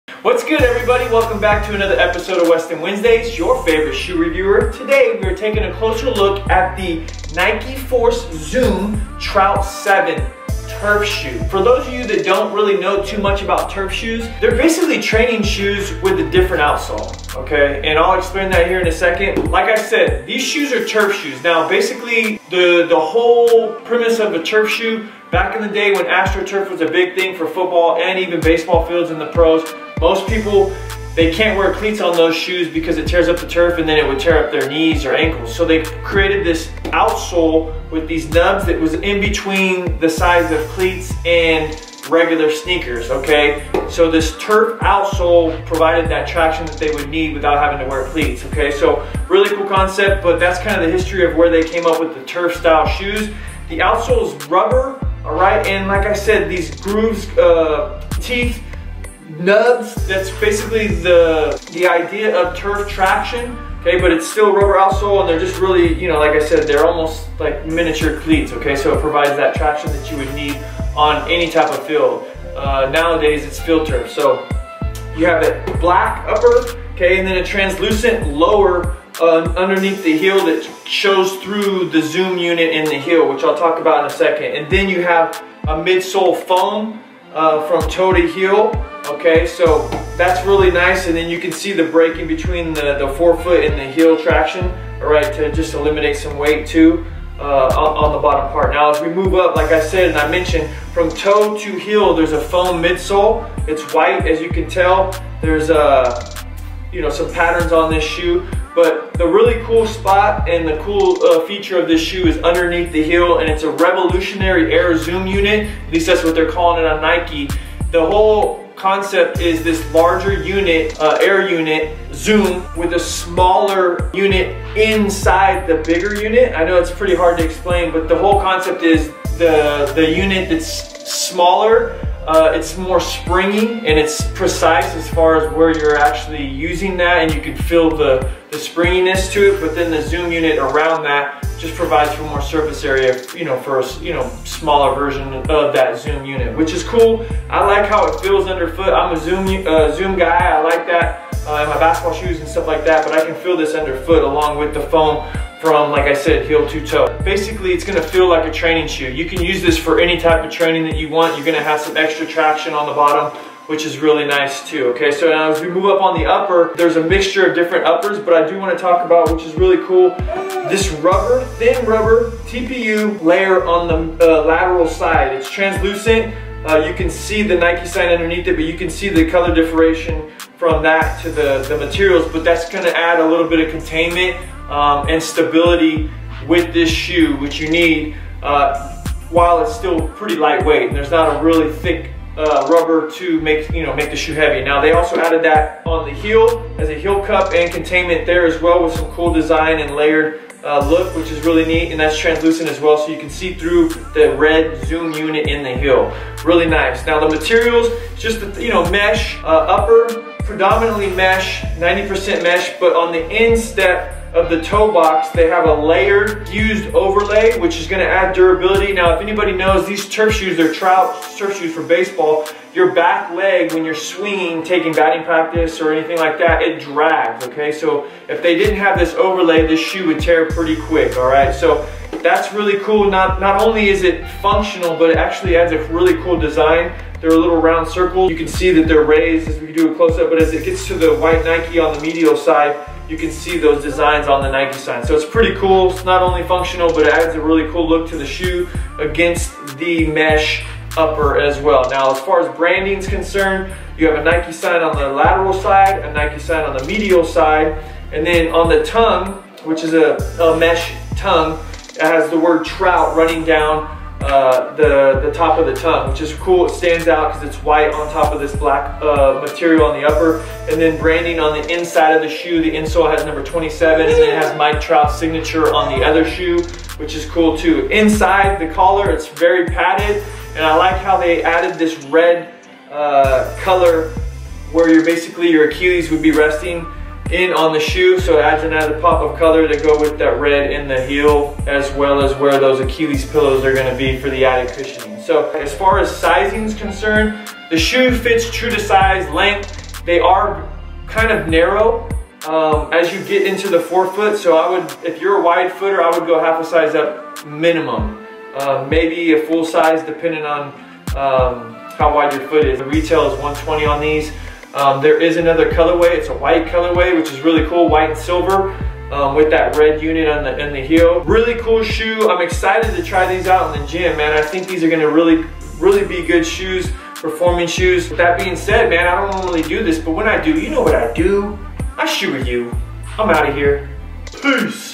Nike Force Zoom Trout 7 Turf Review - On Feet
A turf shoe made for Mike Trout that will perform in the gym as a training shoe for the average joe. A new zoom unit in the heel will give you the impact protection needed and the heel and mid foot cage will give you the stability and containment.